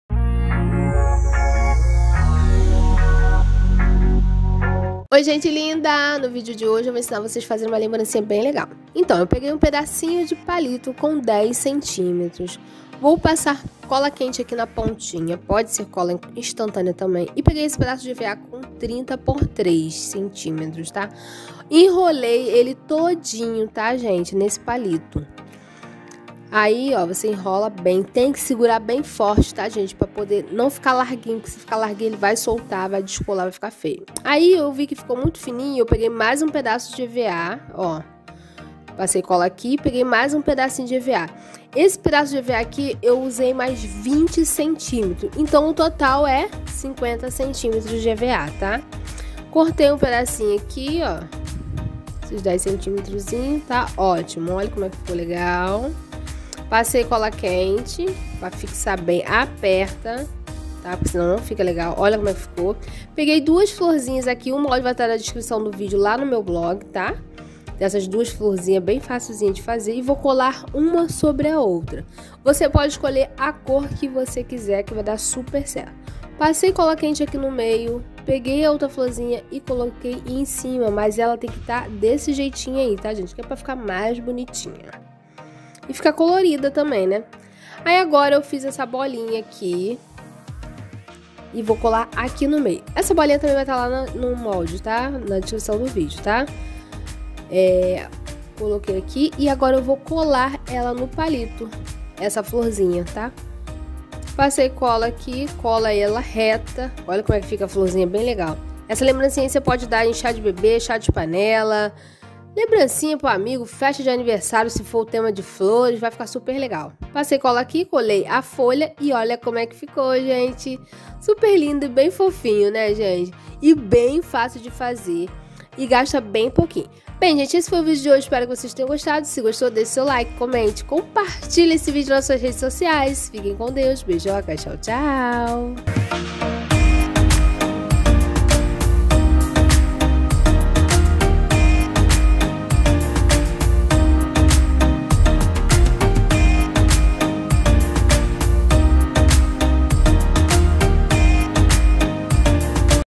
LEMBRANCINHA DE EVA Como fazer FLOR DE EVA | FAÇA VOCÊ MESMO ❤️ Hellen Chagas
LEMBRANCINHA DE EVA Como fazer Flor deEVA | Faça você mesmo
Assista a Playlist Completa:

❤ Gostou? CURTA

MATERIAIS:
- EVA
- Tesoura
- Cola quente ou instantânea
- Palito de churrasco